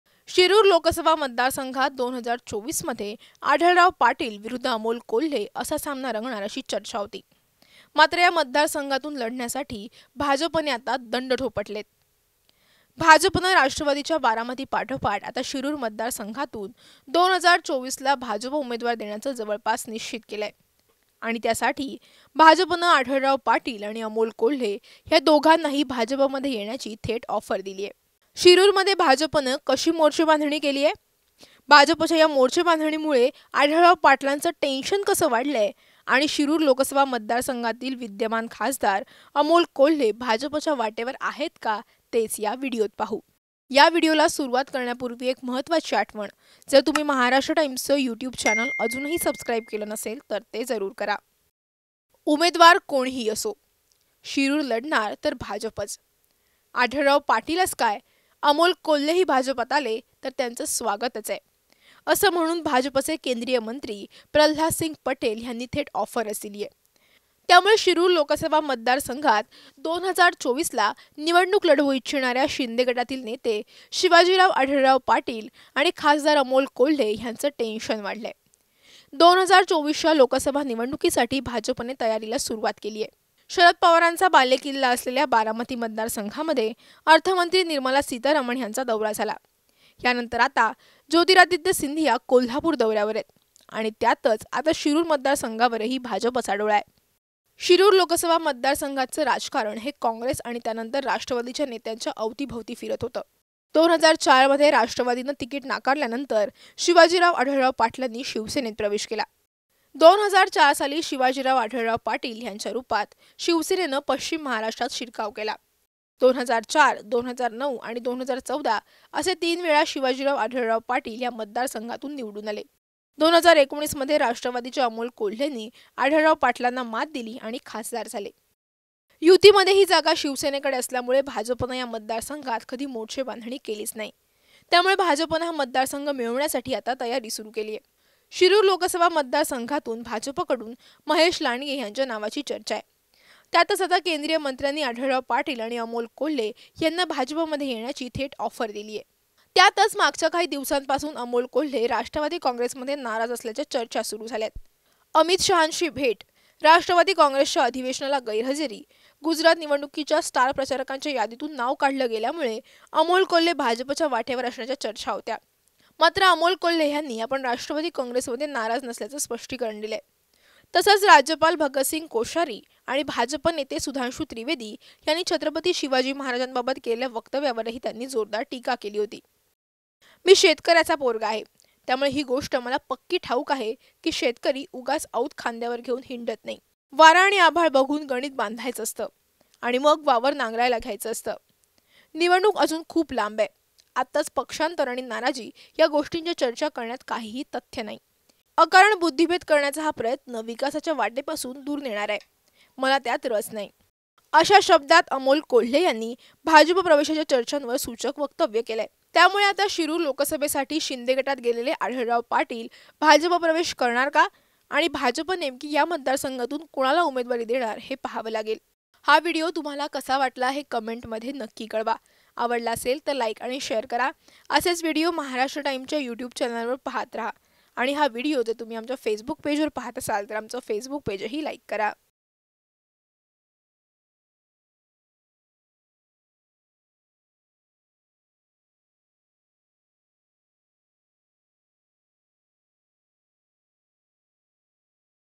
काहीही झालं तरी Shirur Lok Sabha मधून भाजपचा खासदार असेल असं भाजपने जाहीर केलंय | Maharashtra Times
शिरुर लोकसभा मतदारसंघात २०२४ साठी आढळराव पाटील विरुद्ध अमोल कोल्हे असा सामना रंगणार अशी चर्चा होती. मात्र आता या मतदारसंघातून लढण्यासाठी भाजपने दंड थोपटलेत. भाजपनं राष्ट्रवादीच्या बारामती पाठोपाठ आता शिरुर मतदारसंघातून २०२४ ला भाजप उमेदवार देण्याचं जवळपास निश्चित केलंय. शिरुरमध्ये भाजपनं कशी मोर्चेबांधणी सुरु केलीये, भाजपच्या या मोर्चेबांधणीमुळे आढळराव पाटलांचं टेन्शन कसं वाढलंय आणि शिरुर लोकसभा मतदार संघातील विद्यमान खासदार अमोल कोल्हे भाजपच्या वाटेवर आहेत का तेच या व्हिडिओत पाहू...